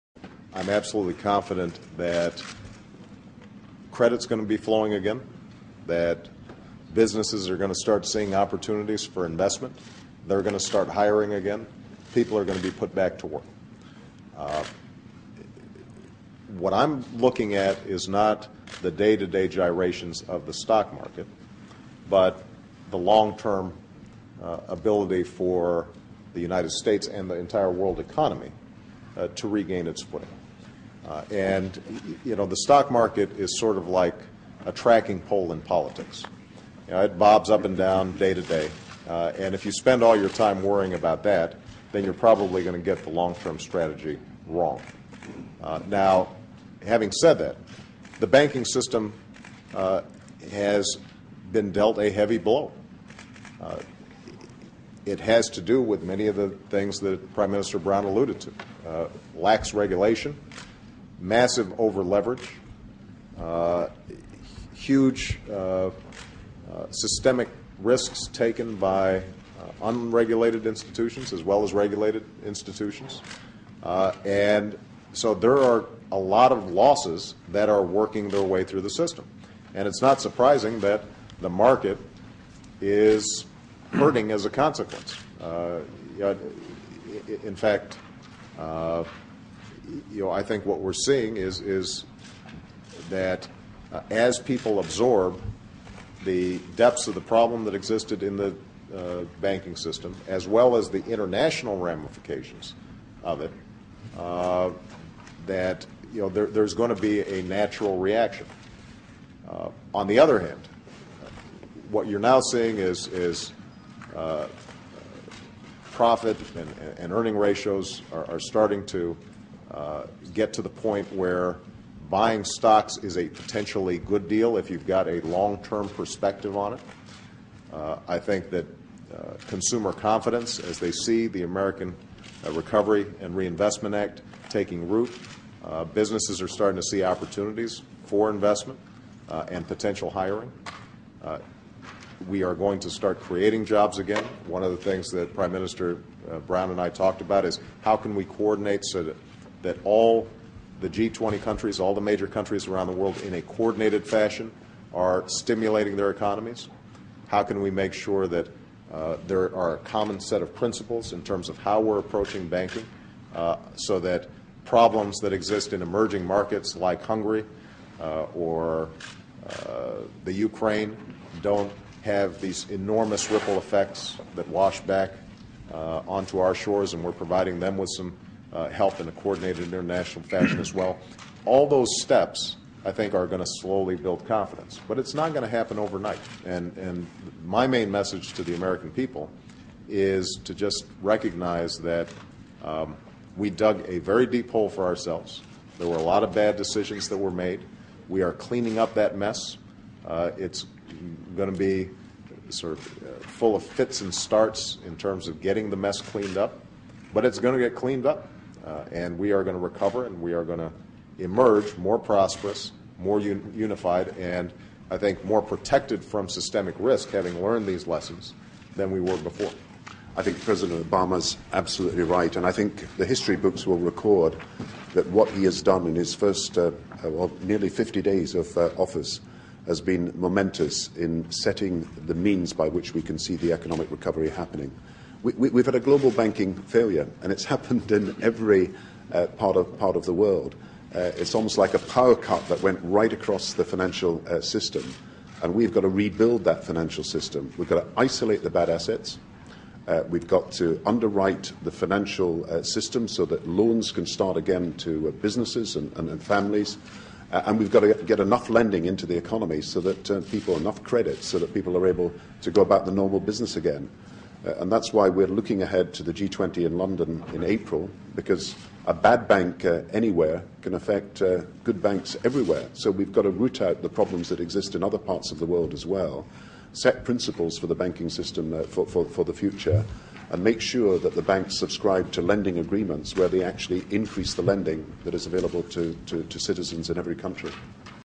BS Artist Obama & Fake Economic Insight (Stock prices "bob like a poll")
Obama "P/E Ratio" Gaffe: Misstatement or Economic Illiteracy? And Will the MSM Report It?

Im not sure whats worse: having our countrys welfare in the hands of a capitalism-hating Marxist ideologue, or having it in the hands of an economic illiterate.

OK, so this morning, President Perfect was discussing the plunging stock market and referred to the profit and earnings ratio. Except its really called Price-to-earnings (P/E) ratio. Whoops.

Granted, I am sure that President Obama has hundreds of figures and terms swirling around in his mind. It goes without saying that being the leader of the free (for now) world will misspeak from time to time.

But, come on. Shouldnt the President—who has put himself personally in charge of the next two generations economic state—be able to utter a pretty basic stock market term like Price-to-Earnings Ratio without bumbling it?

As of this writing, Rush has mentioned it, and so has todays WSJs Best of the Web page.

But will the liberal mainstream Obama-worshiping media mention it tonight?

Will they treat this gaffe with as much giddy mockery as they exhibited every time George W. Bush said nucular or misunderestimated or some other (admittedly comical) malapropism?

Will they do to Obama what they did when Sarah Palin was unable to give Charles Gibson a definition of the Bush Doctrine, even though that terms coiner Charles Krauthammer himself defended Palin by noting there was never one single concrete definition?

No, Im betting the MSM will do what they did when Obama said hes visited 57 states.

And when Obama first called an inhaler a breathalyzer, and then an inhalator.

And when VP Biden said that when the Depression happened [in 1929], FDR went on TV and explained it to the American people.

Hardly anything.